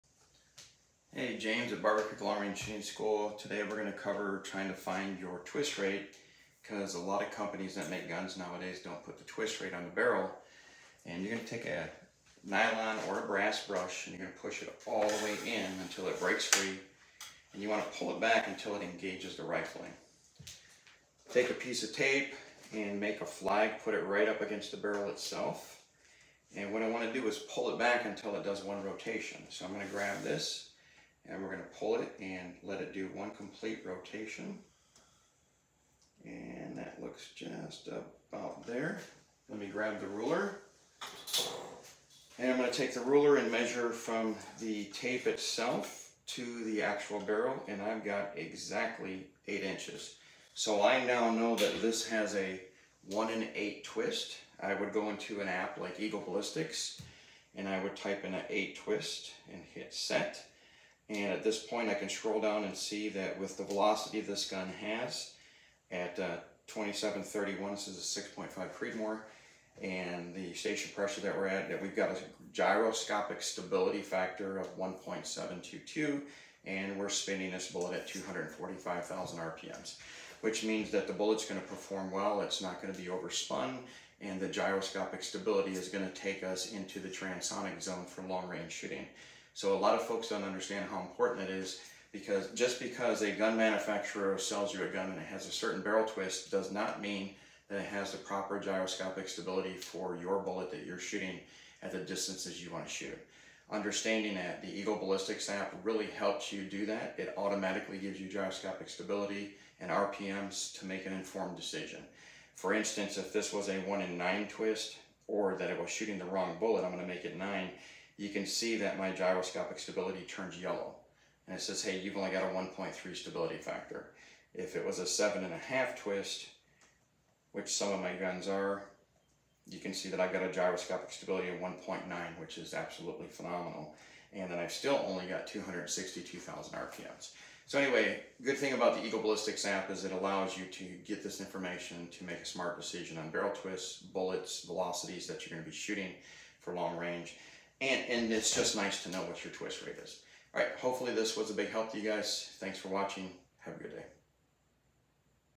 James Eagleman at Barbourcreek Long range Shooting School goes over finding your barrel twist
The importance of gyroscopic stability and barrel twist ￼